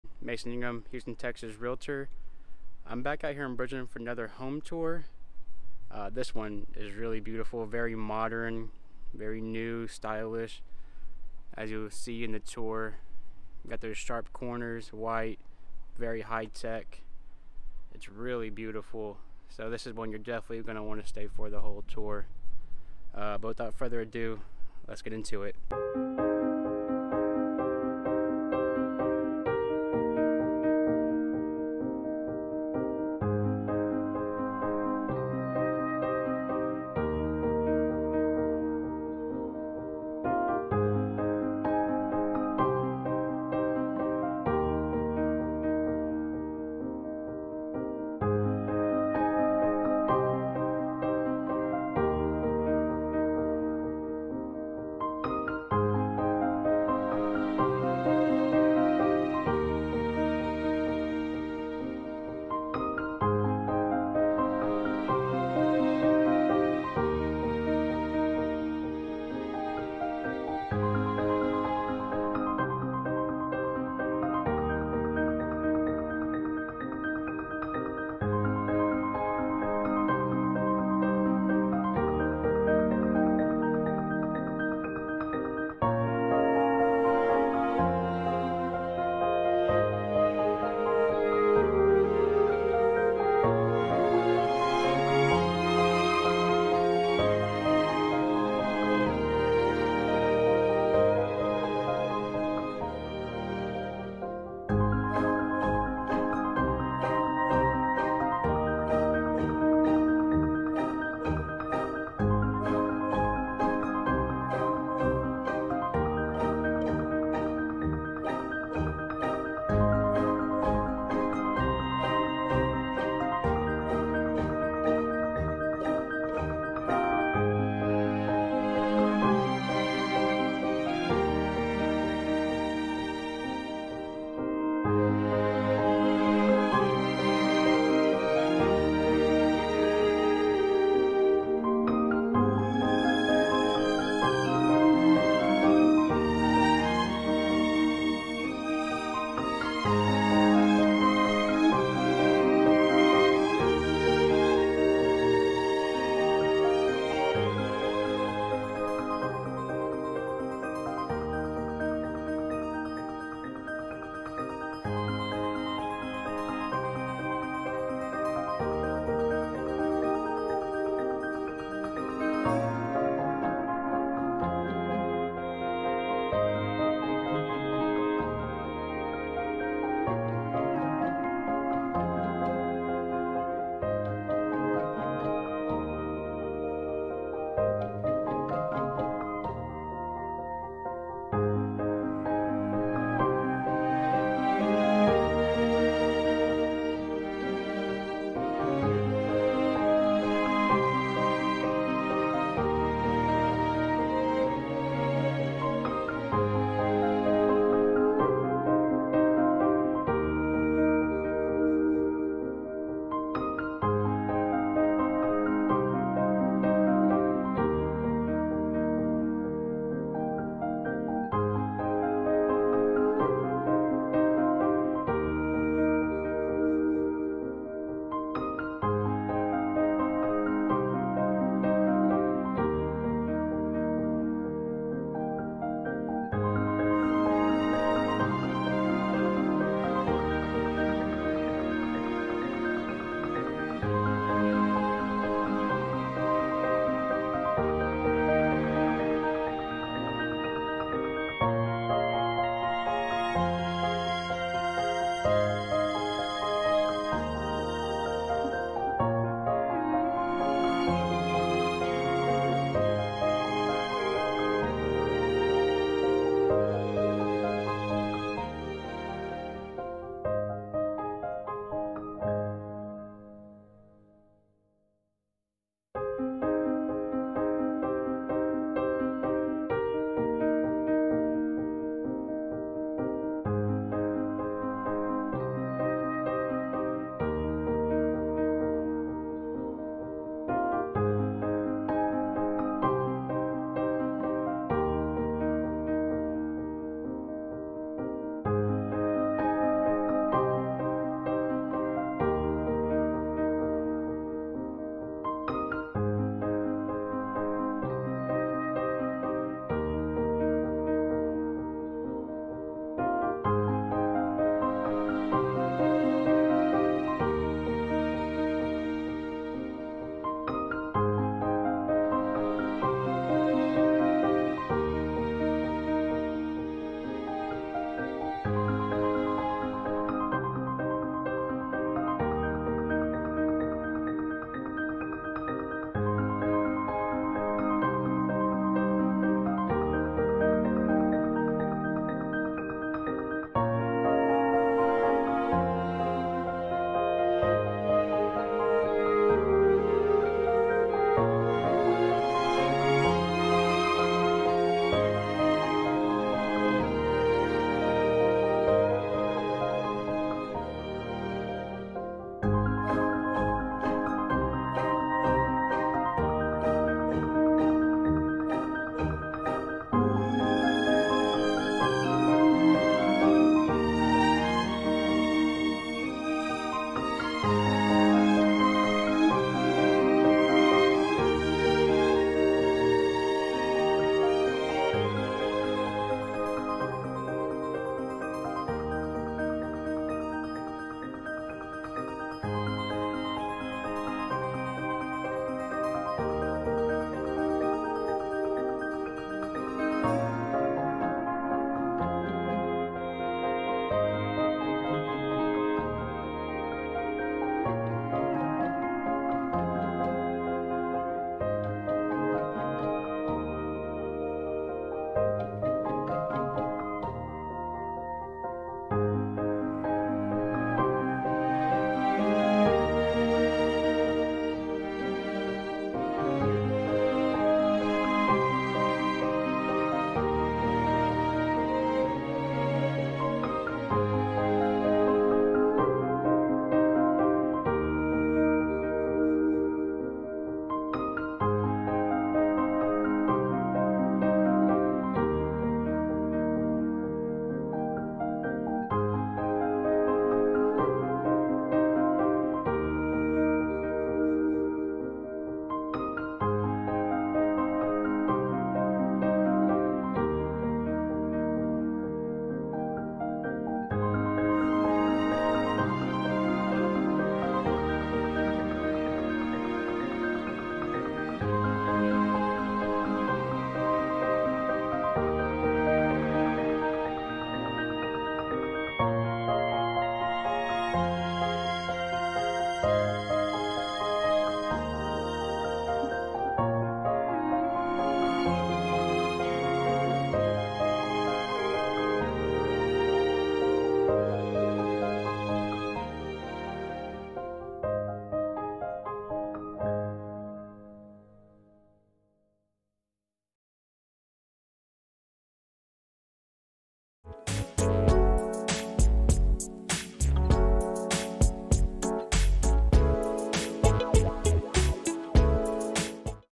CUSTOM LUXURY MODEL HOME IN THE BRIDGELAND COMMUNITY | Cypress, TX | $1,600,000+
Beautiful custom luxury home in the sought after community of Bridgeland. Bridgeland is located in in the North West region of Houston. Growing day by day Bridgeland is a top community in all of the Houston are and is a staple in the Cypress, Texas area. Zoned to CFISD Bridgeland offers a wide range of amenities for the residents and is great for families! Check out this tour of a model home in Bridgeland (Cypress, TX)

Priced From 900’s - $1.5mil+

(Pricing and Availability is subject to change without notice especially due to the current Real estate market)

Videos shown are model homes, pricing to replicate the model houses will have to be discussed with builders.

Pricing will vary depending on your floor plan selection, upgrades, lot size, etc. Pricing is available upon request for the builders.